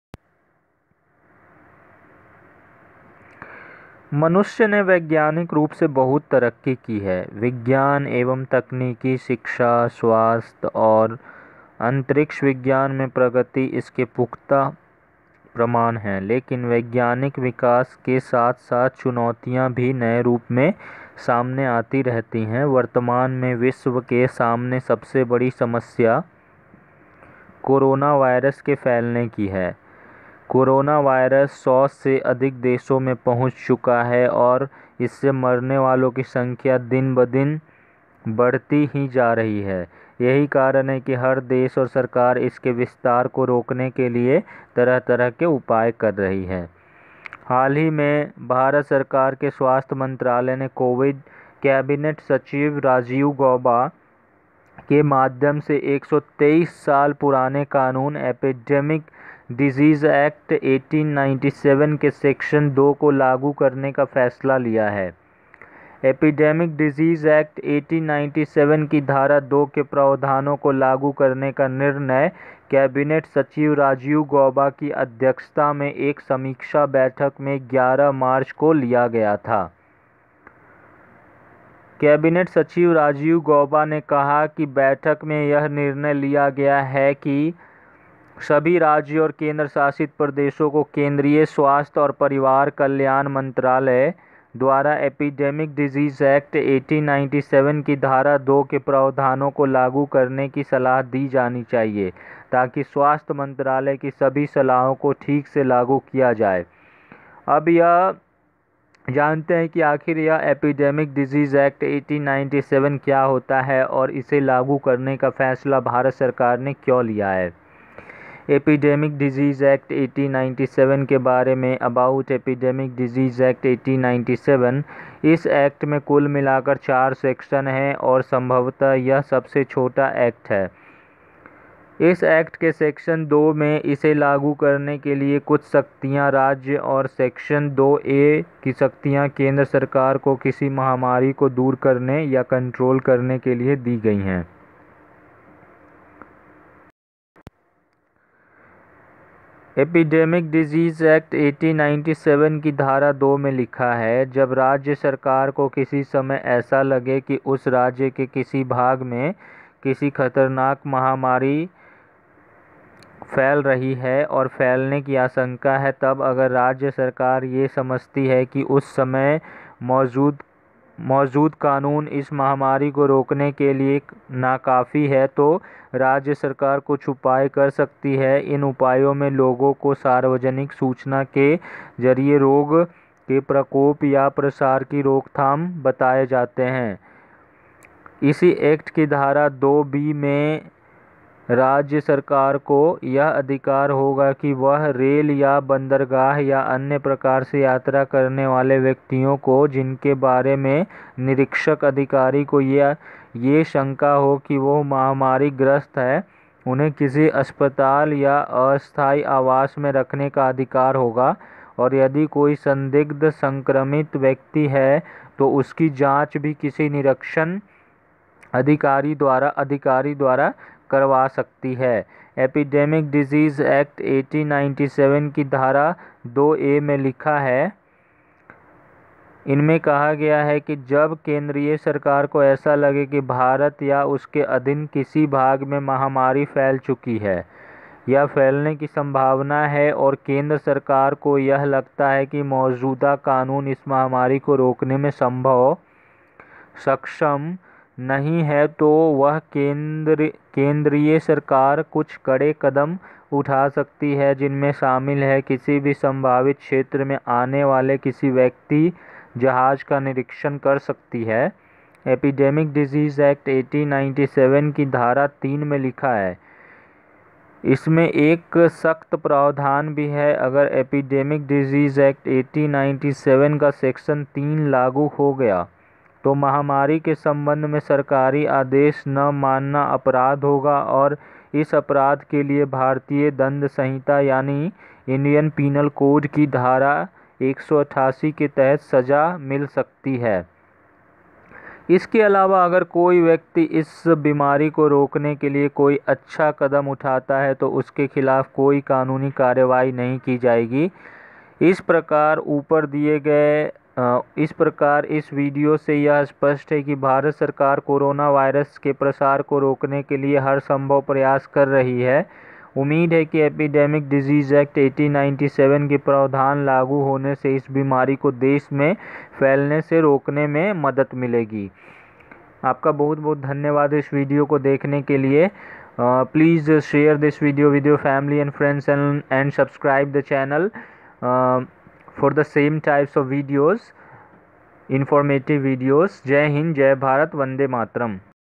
Know!! About Epidemic Disease Act, 1897
मनुष्य ने वैज्ञानिक रूप से बहुत तरक्की की है. विज्ञान एवं तकनीकी, शिक्षा, स्वास्थ्य, और अन्तरिक्ष विज्ञान में प्रगति इसके पुख्ता प्रमाण हैं.लेकिन वैज्ञानिक विकास के साथ साथ चुनौतियाँ भी नए रूप में सामने आतीं रहतीं हैं.वर्तमान में विश्व के सामने सबसे बड़ी समस्या कोरोना वायरस के फैलने की है.

कोरोना वायरस 100 से अधिक देशों में पहुँच चुका है और इससे मरने वालों की संख्या दिन व दिन बढती ही जा रही है.यही कारण है कि हर देश और सरकार इसके विस्तार को रोकने के लिए तरह तरह के उपाय कर रही है.